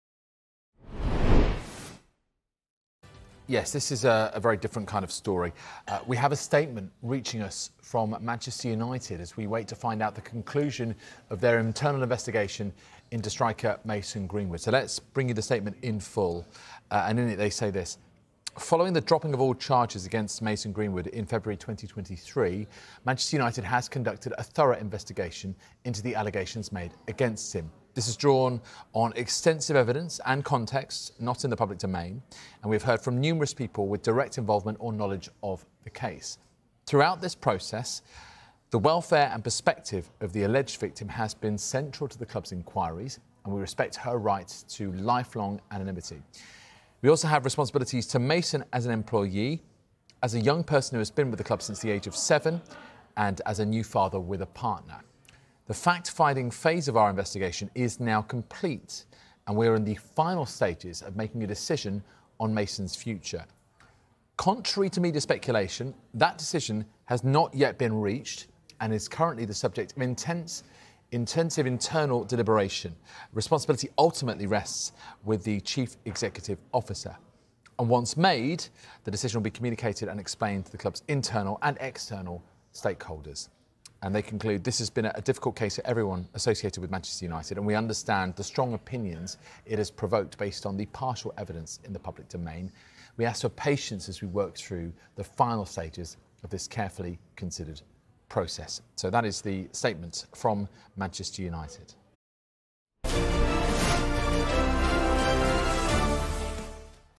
Manchester United in 'final stages' of making decision on Mason Greenwood
After all charges against Mason Greenwood were dismissed in February 2023, Manchester United have since carried out a comprehensive inquiry into the accusations brought against the former player.